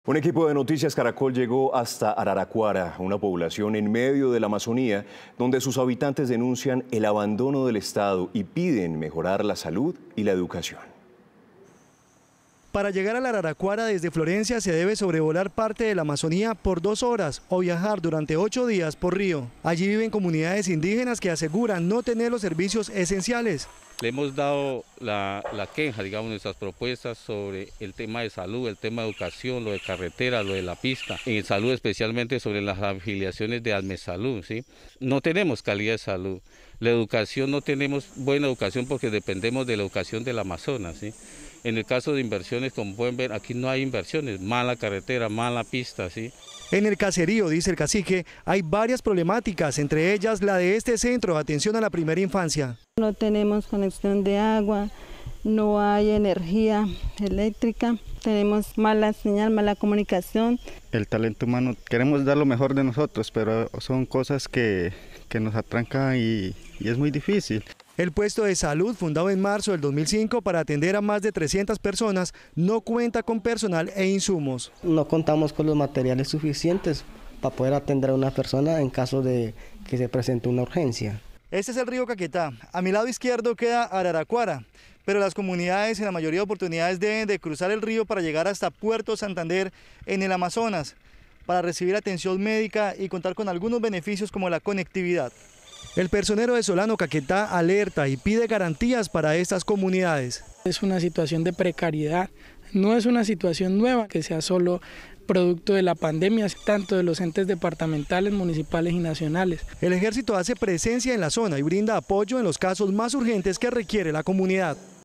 Resguardo indígena de Araracuara no tiene salud ni educación ante abandono por parte del Estado
Noticias Caracol llegó hasta Araracuara, una población en medio de la Amazonía, donde sus habitantes denuncian el abandono del Estado colombiano, piden mejorar la salud y la educación.

Allí habitan comunidades indígenas que aseguraron no tener los servicios esenciales para vivir de una manera digna.

Las comunidades de Araracuara deben cruzar el Río Caquetá y llegar hasta Puerto Santander para tener atención médica y conectividad.

El Ejército Nacional hace presencia en la zona y brinda apoyo en los casos más urgentes que requiere la comunidad.